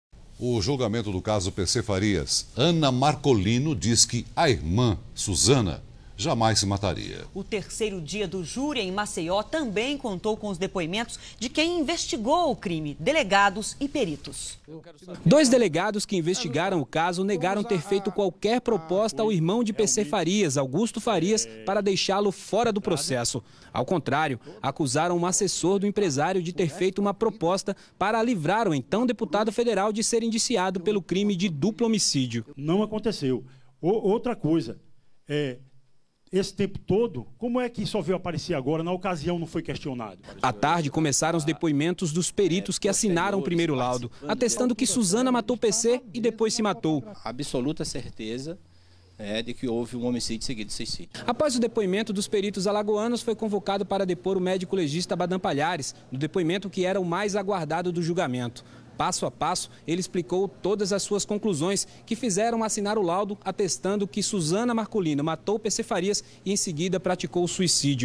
3º dia do julgamento PC Farias tem depoimentos de investigadores do caso
Os peritos que assinaram o primeiro laudo do crime afirmaram que Suzana matou PC Farias e, depois, se suicidou. A irmã de Suzana disse que ela nunca se mataria.